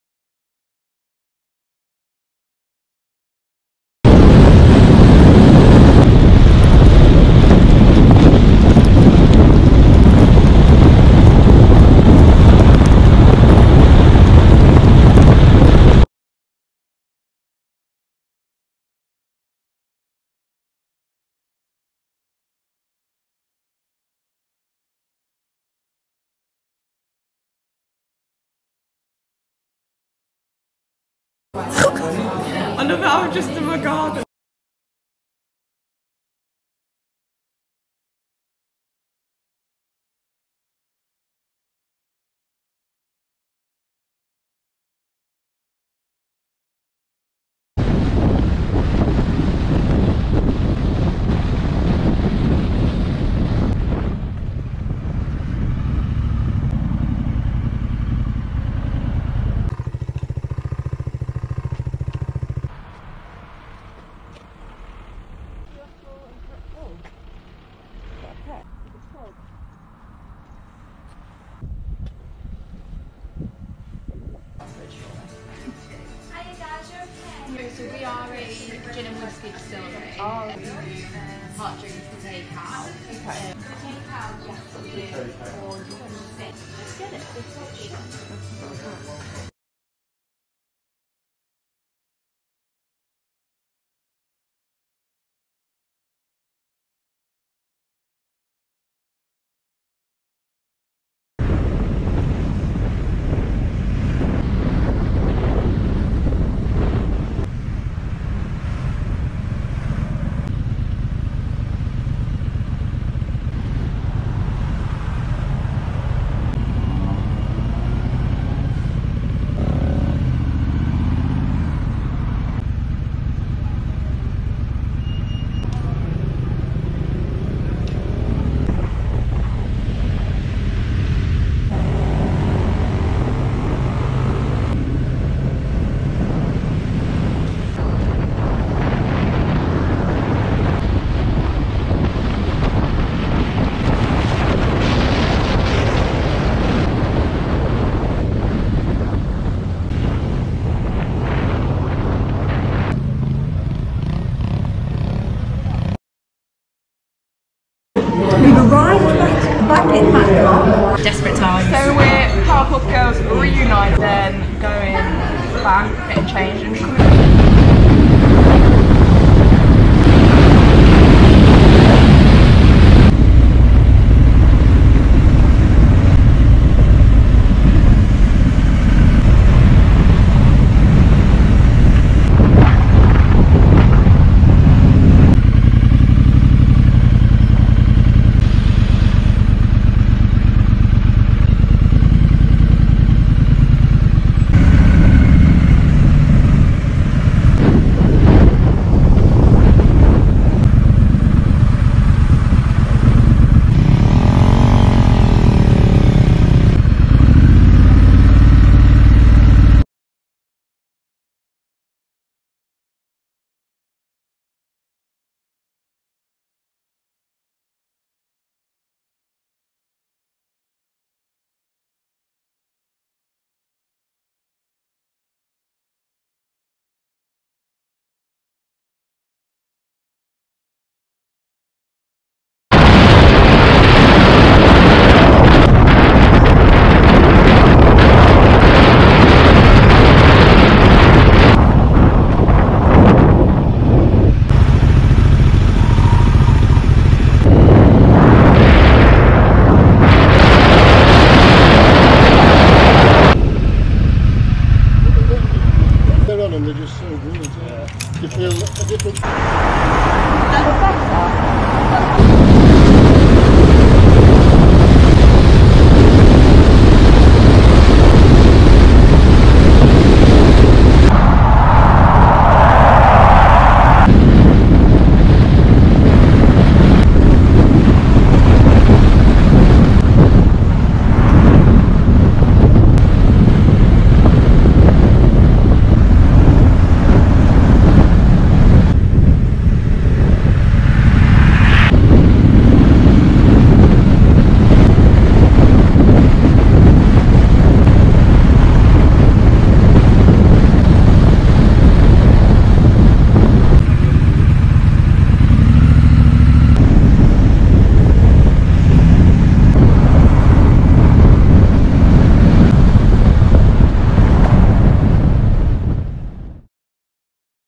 Girls Motorbike Trip To MATLOCK
Join us on our first UK trip to the Peak District

Featuring some questionable GoPro skills, food, drink, meeting up with our friend and of course, some classic British weather!

​@Official Triumph Motorcycles   @Herald Motor Co.   @Honda Automóveis